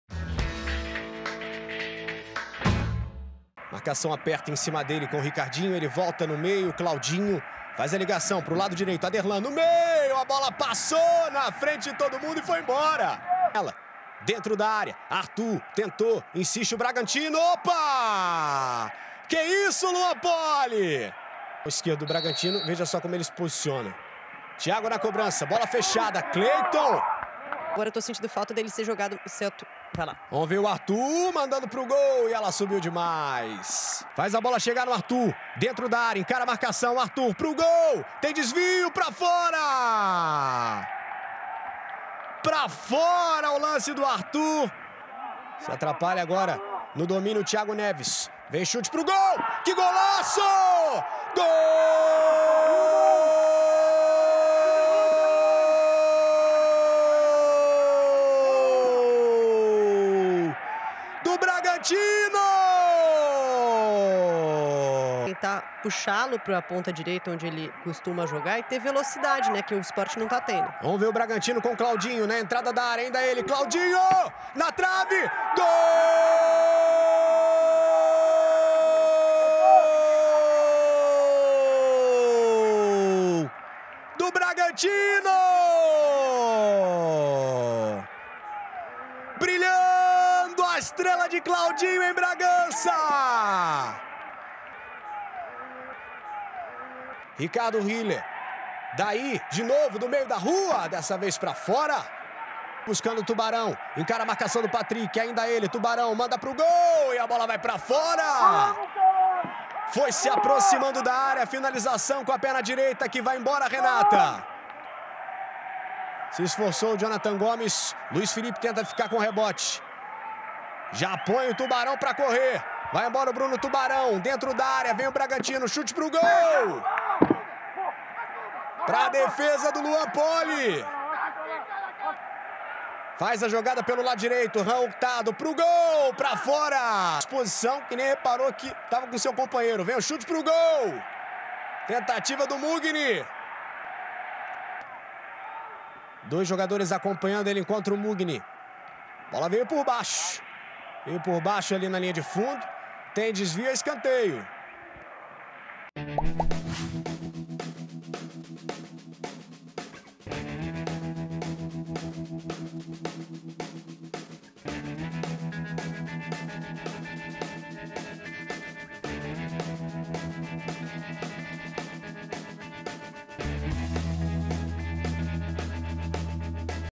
BRAGANTINO 2 X 0 SPORT | MELHORES MOMENTOS | 17ª RODADA BRASILEIRÃO 2020 | ge.globo
Bragantino volta a vencer após cinco jogos, respira e amplia má fase do Sport. Time paulista segue na vice-lanterna da Série A, mas embola classificação com 16 pontos e fica mais perto de sair do Z-4; Leão acumula quarta derrota seguida e segue caindo, agora em 11º, com 20.